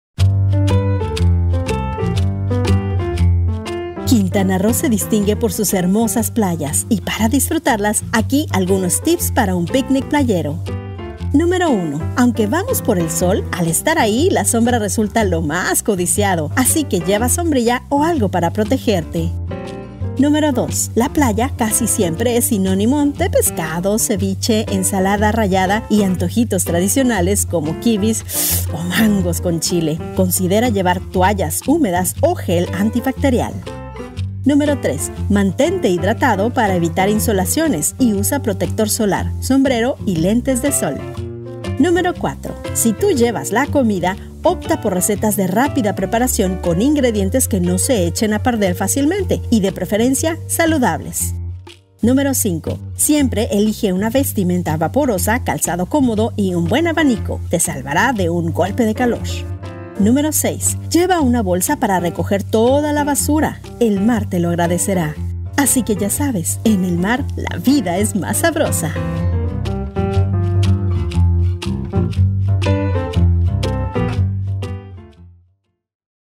Tips para un picnic playero | El mundo en un bocado
Quintana Roo es uno de los estados de México, con la mayo cantidad de playas y sobre todo, playas con Blue flag, de México.

En este sentido y festejando los 45 años de ser un estado libre y soberano, te compartimos algunos tips para que pases el mejor picnic playero que puedas tener, en compañía de quien más gustes.